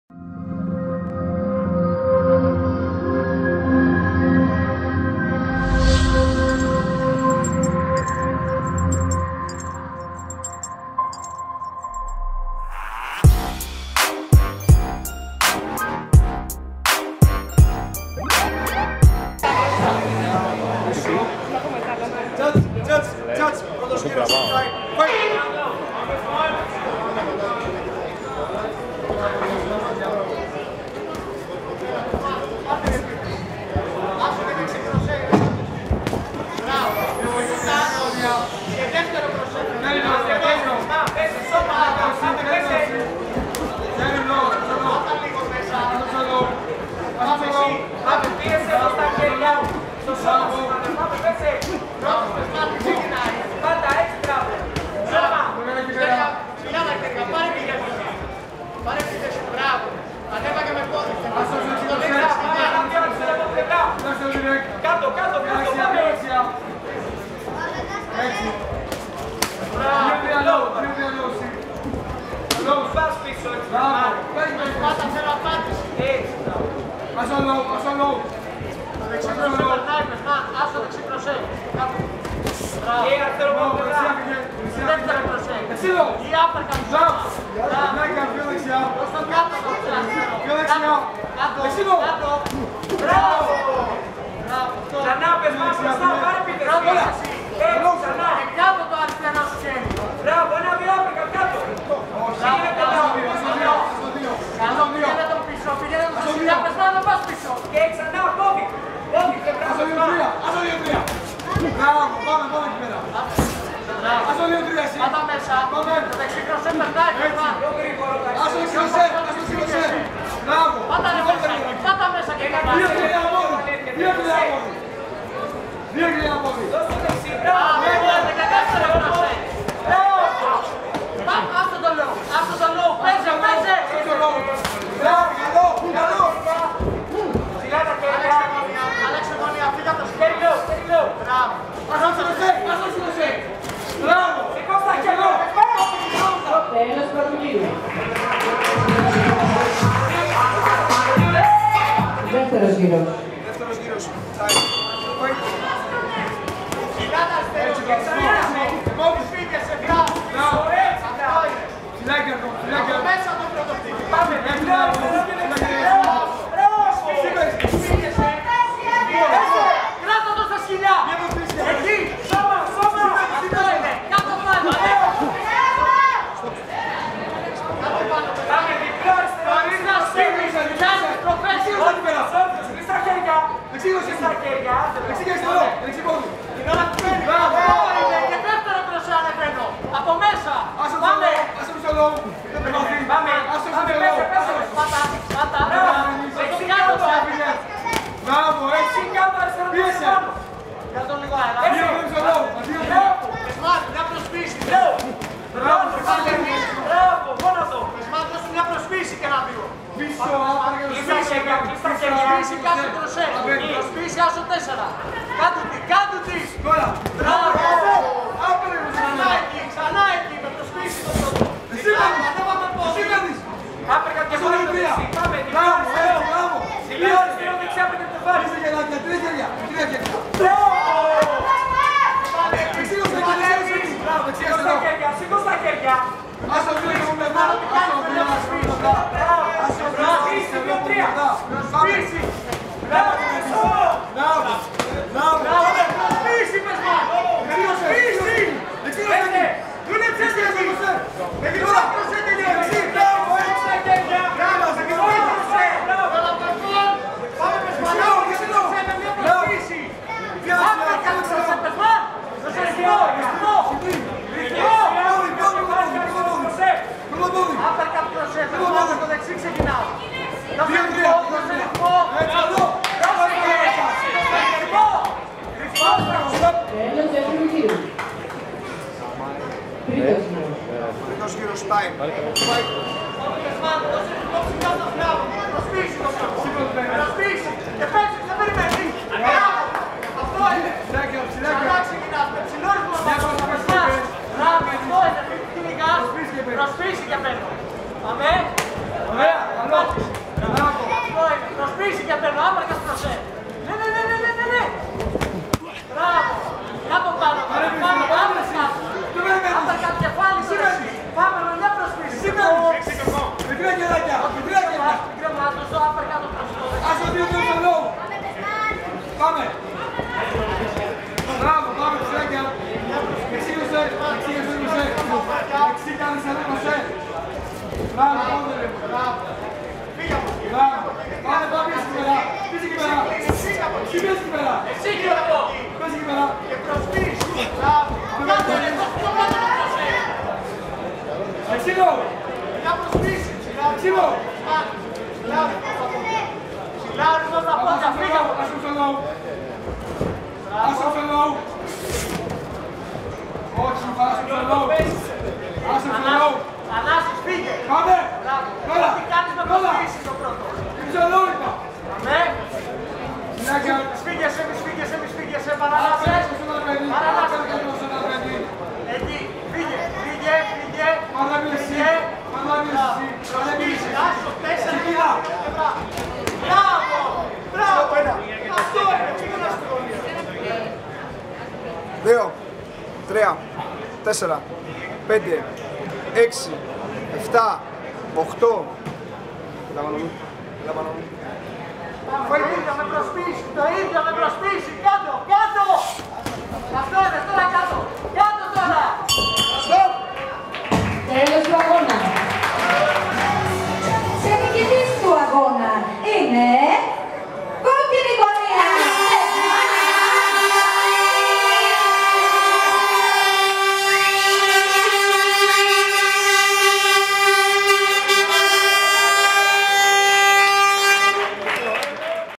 Versus 7/SF1: Pezhman Tafti vs Γιώργος Κόντρα
Pezhman Tafti (FIGHTERS ATHANASOPOULOS) vs Κόντρα Γιώργος (FIGHTERS ATHANASOPOULOS)
Νικητής ο Pezhman Tafti